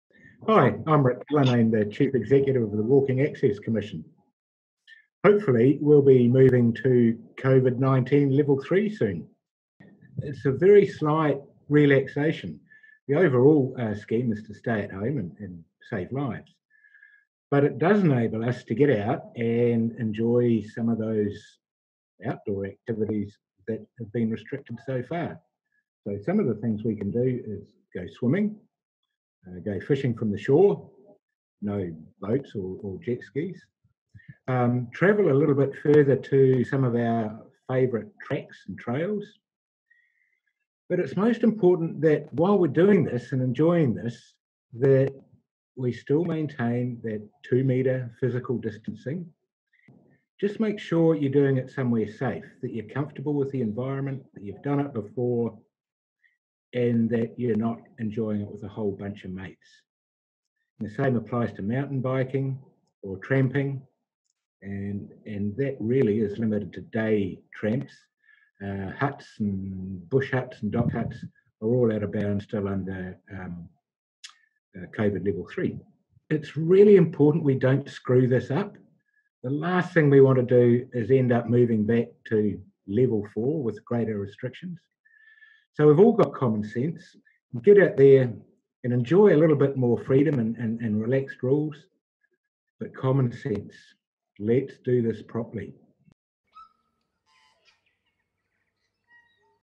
Ric Cullinane talks about outdoor activities under COVID-19 Alert Level 3
When we go to Alert Level 3 the plan is still to stay at home and save lives. But it does enable us to get out and enjoy some of those outdoor activities that have been restricted so far.